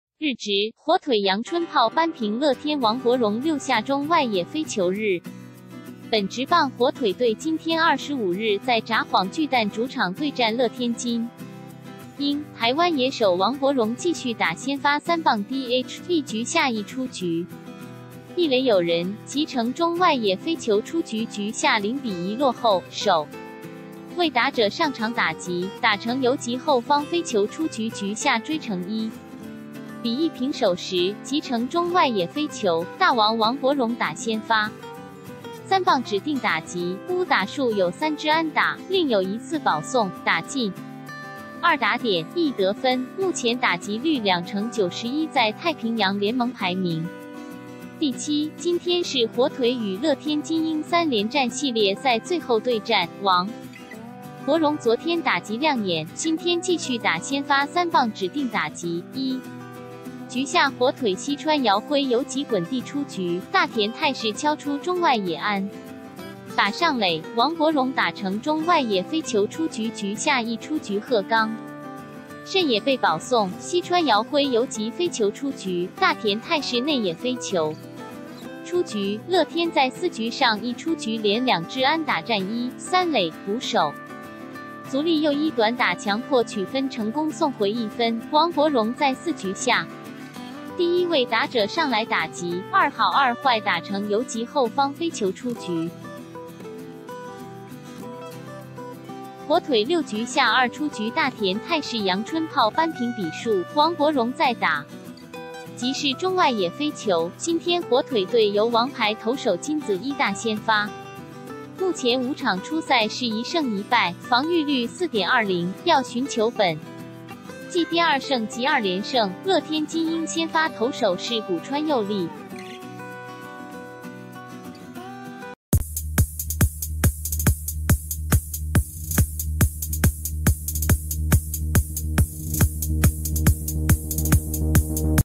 日職／火腿陽春砲扳平樂天 王柏融6下中外野飛球
日本職棒火腿隊今天（25日）在札幌巨蛋主場對戰樂天金鷹，台灣野手王柏融繼續打先發3棒DH，1局下一出局一壘有人，擊成中外野飛球出局。4局下0比1落後，首位打者上場打擊，打成游擊後方飛球出局。6局下追成1比1平手時，擊成中外野飛球。

「大王」王柏融打先發3棒指定打擊，5打數有3支安打，另有1次保送，打進2打點、1得分，目前打擊率2成91在太平洋聯盟排名第7。今天是火腿與樂天金鷹3連戰系列賽最後對戰，王柏融昨天打擊亮眼，今天繼續打先發3棒指定打擊。

1局下火腿西川遙輝游擊滾地出局，大田泰示敲出中外野安打上壘，王柏融打成中外野飛球出局。3局下一出局鶴岡慎也被保送，西川遙輝游擊飛球出局，大田泰示內野飛球出局。樂天在4局上一出局連2支安打占一、三壘，捕手足立祐一短打強迫取分成功送回1分。

王柏融在4局下第一位打者上來打擊，2好2壞打成游擊後方飛球出局。火腿6局下二出局大田泰示陽春砲扳平比數，王柏融再打擊是中外野飛球。

今天火腿隊由王牌投手金子弌大先發，目前5場出賽是1勝1敗、防禦率4.20，要尋求本季第2勝及2連勝。樂天金鷹先發投手是古川侑利。